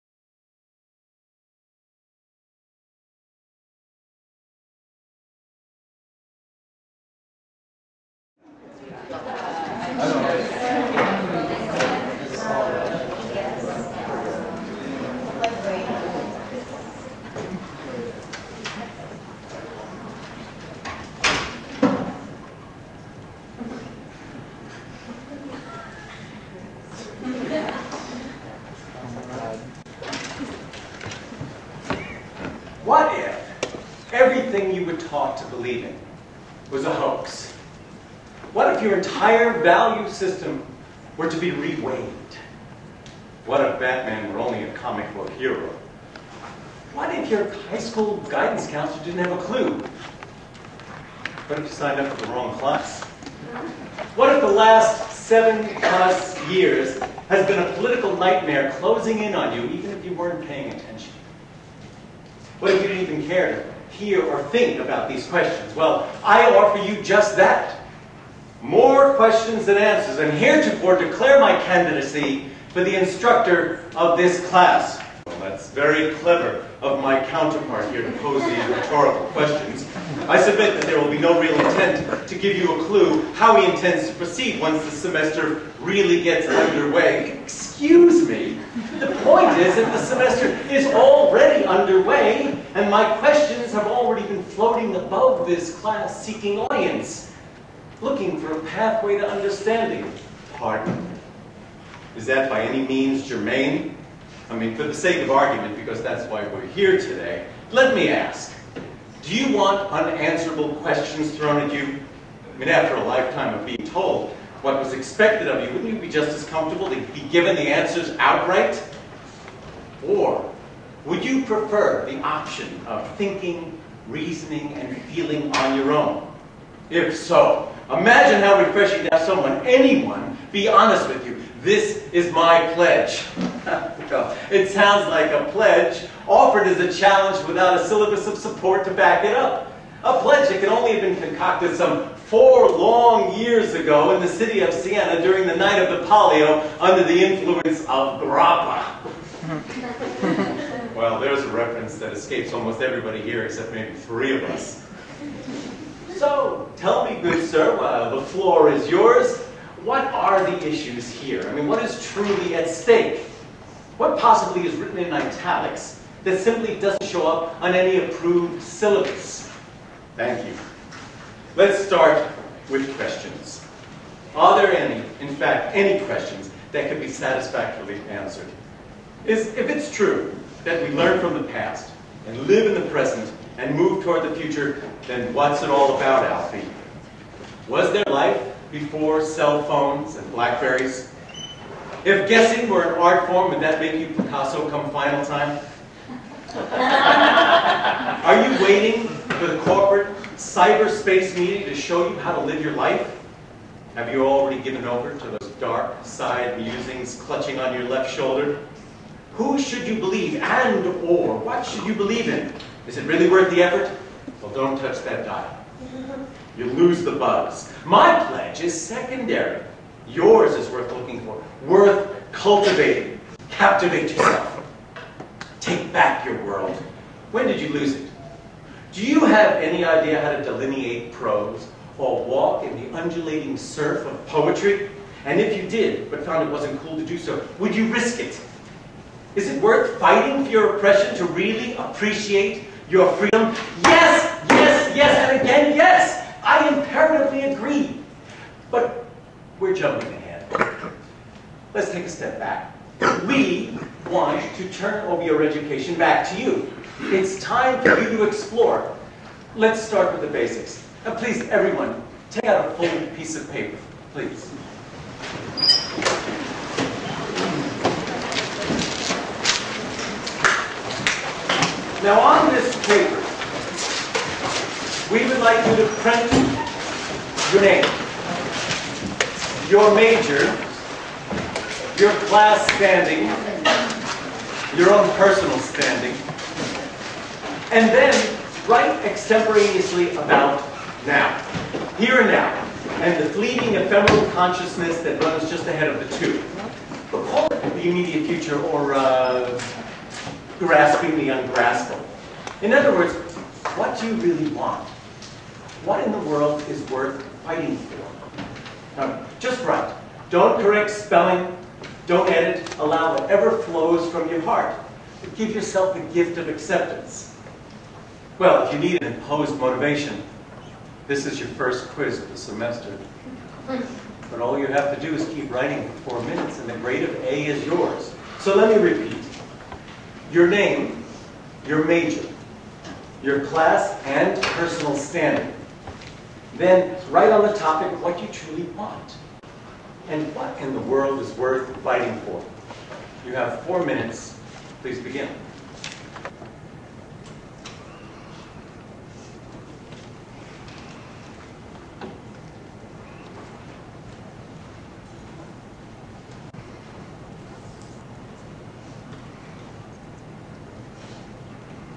Martin Katz Theater 100 Opening Ceremony: Part 1
The opening ceremony for Matin Katz's Theater 100 class. SDSU 2008

Filmed and Edited by Demitri Andrikopoulos